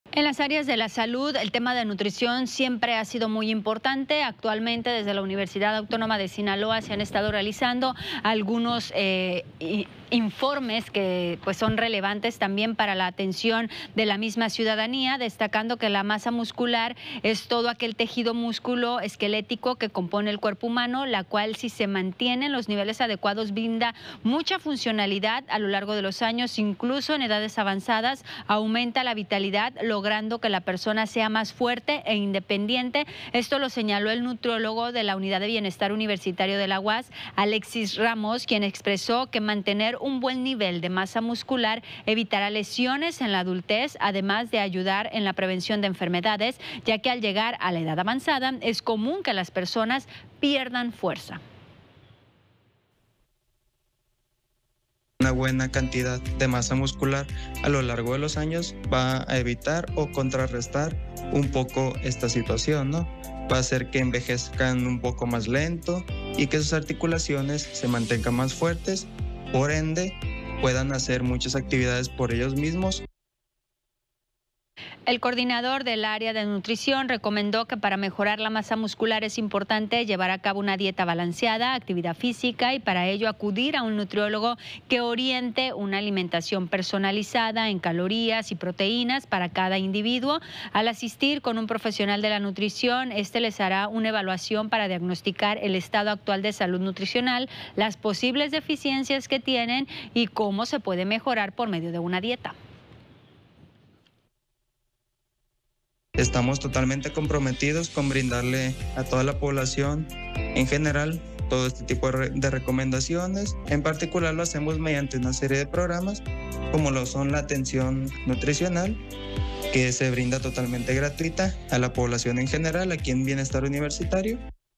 UAS habla de la importancia de la nutrición y ejercicio para la vejez | Las Noticias TVP
UAS realiza informes sobre nutrición y la importancia de mantener una buena musculatura para evitar problemas de salud en la vejez.